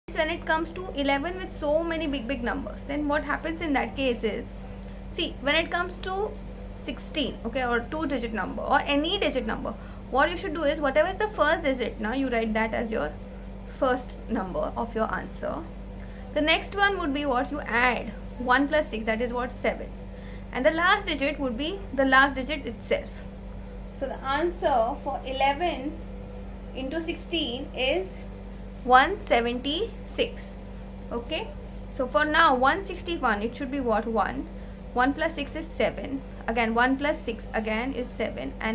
A Brilliant Multiplication trick!
Hi, this is a vedic math technique where in you can do multiplication in secs!!

You can also check out my video on use of MATHEMATICS in REAL LIFE on the following link: